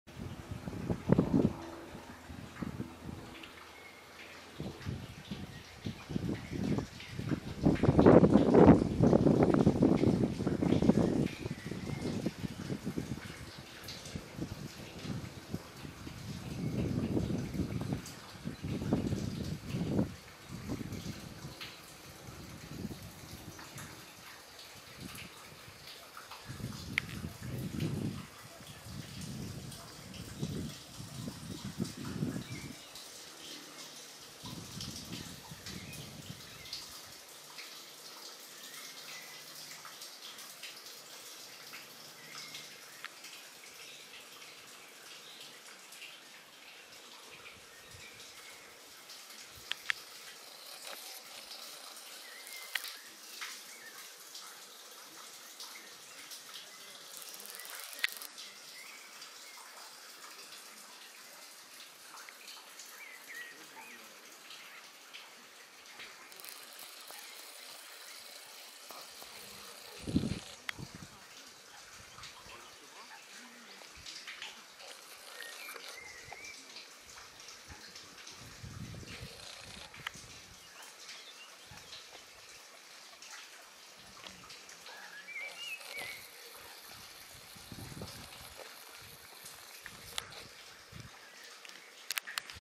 L'étang de Brin version mai 2014
Une balade nature au bord de l'étang de Brin. C'est à Champenoux ( Meurthe et Moselle, 54).
C'est une détente agréable.